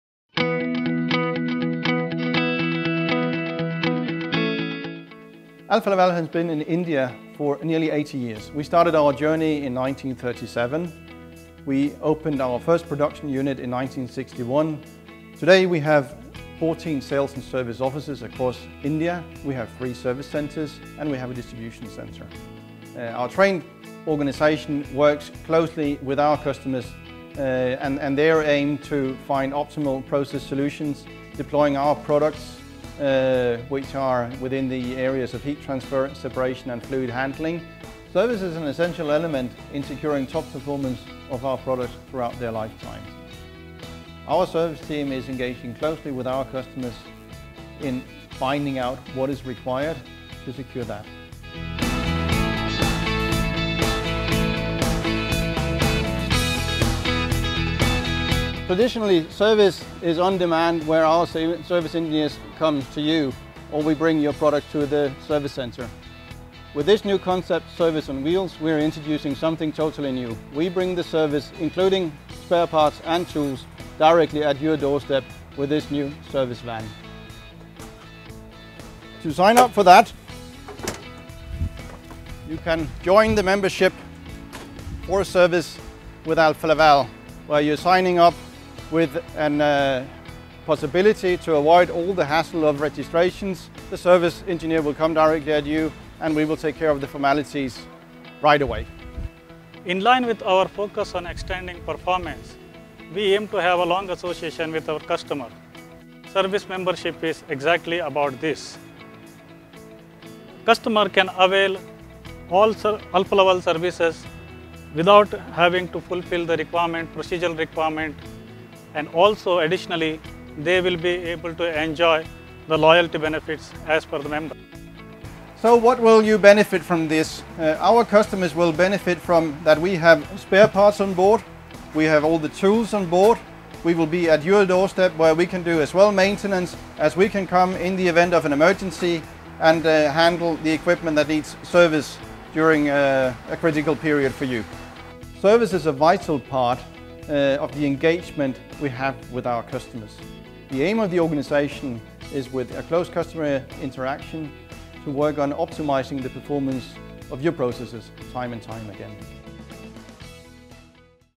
Introducing Service on Wheels
Alfa Laval’s history in India goes back to 1937. There are currently about 20 Alfa Laval locations, including sales offices, distribution centers and service centers. Now, Alfa Laval India is introducing a new concept for helping the customers with their Alfa Laval products. Called Service on Wheels, it enables Alfa Laval India to offer on-site service in a more efficient way. Service technicians will arrive at the customer site in a van, packed with all the tools and spare parts that could be necessary. The Service on Wheels concept covers both maintenance and emergency support. Those that sign up for the program will enjoy various loyalty benefits that will further ensure the top performance of their Alfa Laval equipment.